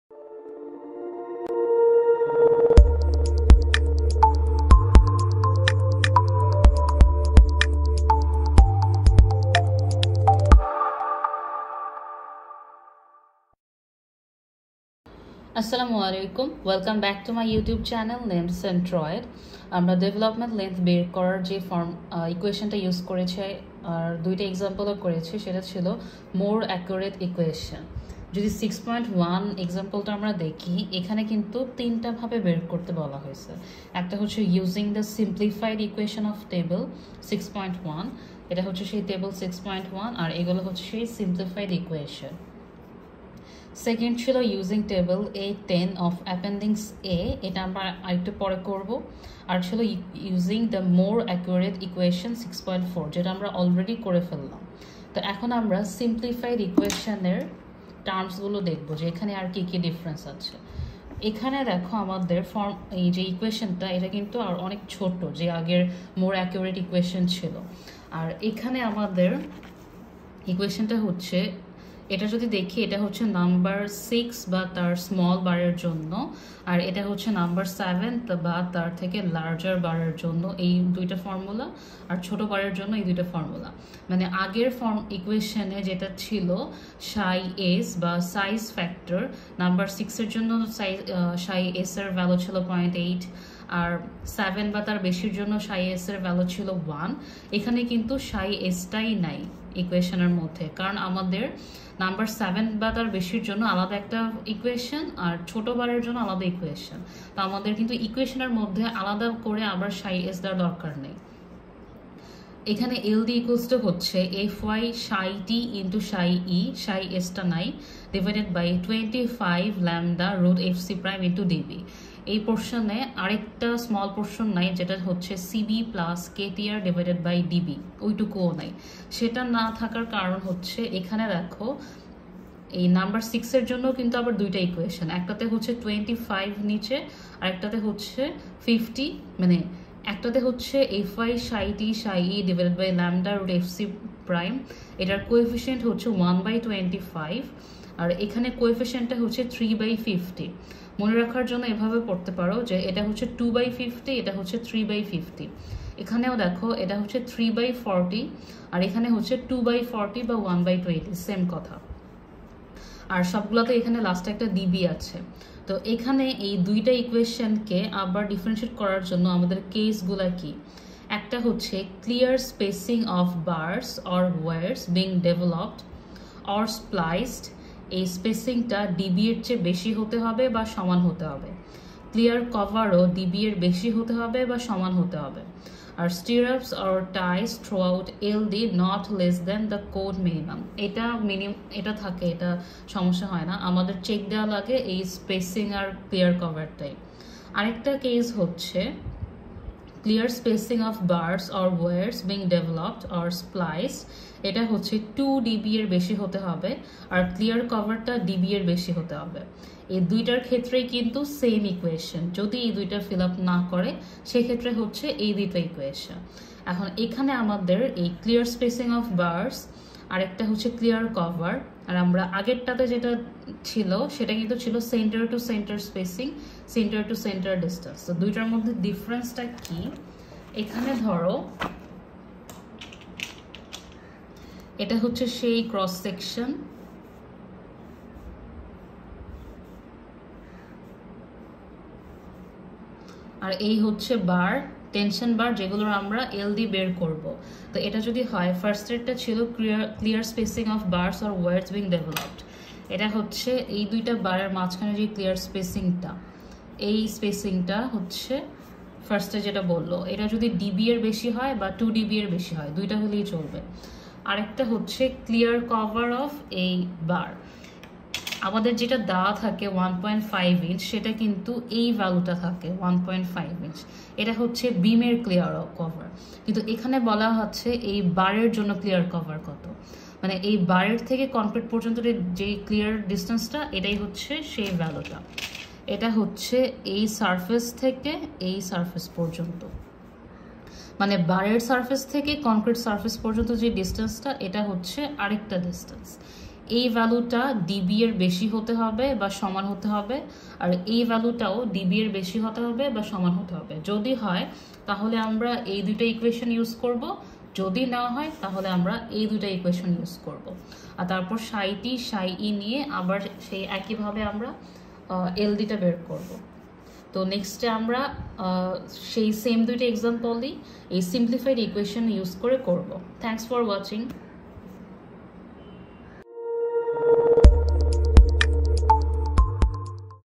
4. DETERMINING DEVELOPMENT LENGTH || SIMPLIFIED EQUATION || BASIC CONCEPT || Bangla Tutorial
This tutorial comprises an easy and detailed explanation of the basic concepts of determining Development Length. The tutorial is made by Noorjahan Emrona Emu, one of the top students from the Civil Engineering Department of the Military Institute of Science & Technology (MIST).

Editing Credit🎬: Muhaimenul Islam Mohim.